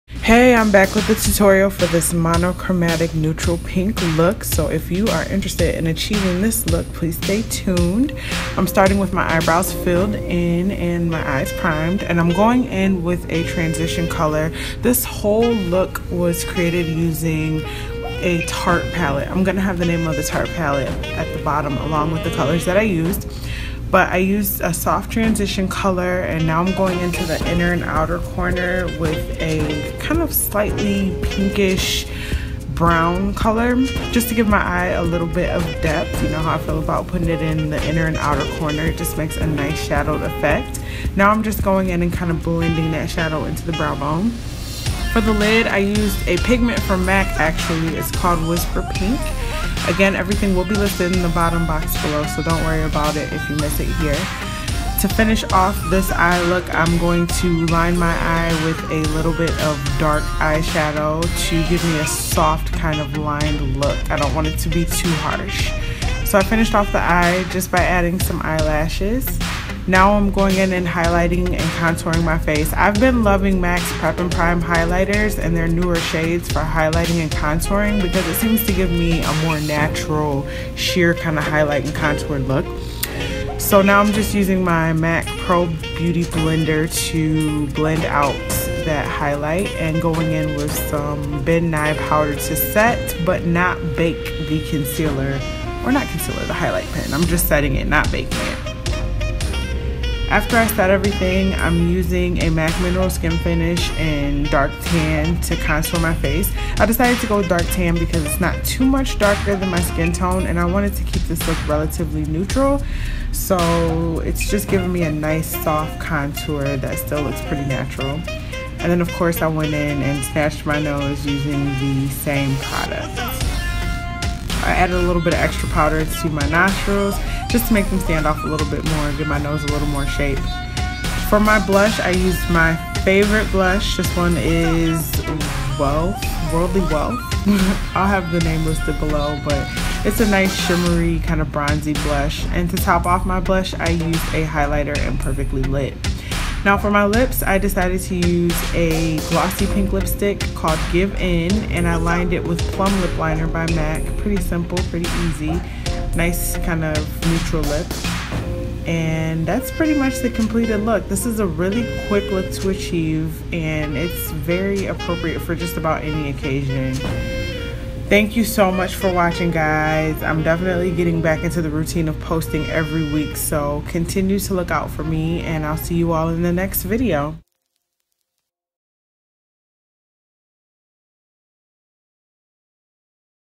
Monochromatic Neutral Pink Full Face Tutorial
Products

Face:

MAC Studio Finish Concealer NW 40
MAC Mineral Skinfinish Medium Tan
MAC Studio Sculpt Foundation NW 43
MAC Prep and Prime Higlighter Dark
MAC Mineral Skinfinish Dark Tan
MAC Worldly Wealth Blush
MAC Perfectly Lit Mineral Skinfinish

Eyes:

Tartelette in Bloom Palette Smarty Pants, Rebel and Smokeshow
MAC Whisper Pink Pigment

Lips:

MAC Lipstick Give In
MAC Lip Pencil Plum